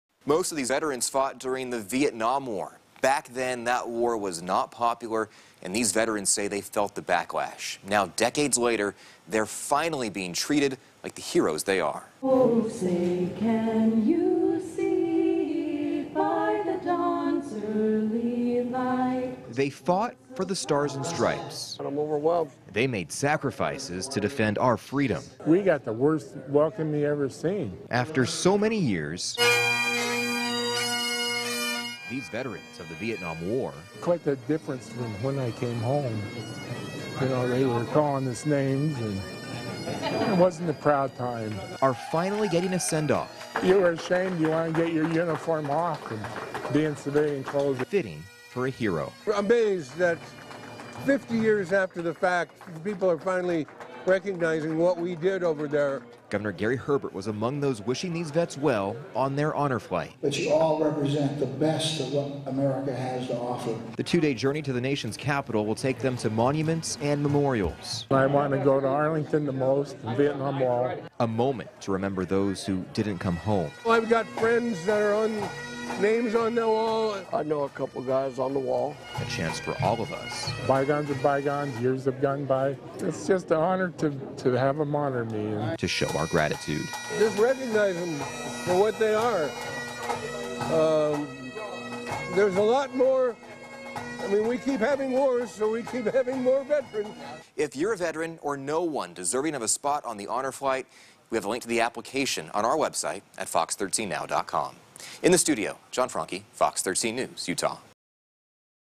Veterans take Honor Flight to DC
Dozens of Utah’s military veterans are finally getting the recognition they deserve.